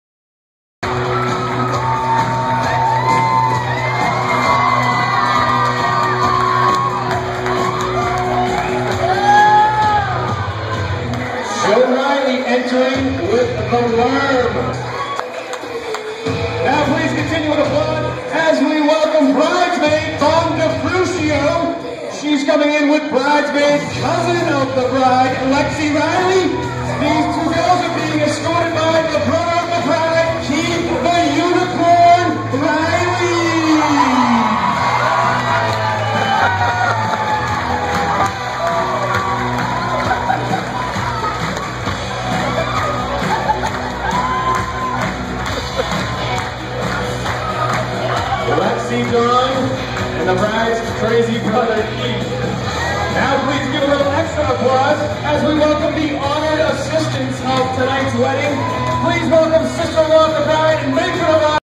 Unicorn Bridal Party Reception Entry - Best Ever
The best bridal party entry ever. After one of my nephew's does the worm, my other nephew comes in with a unicorn head on. Hysterically funny.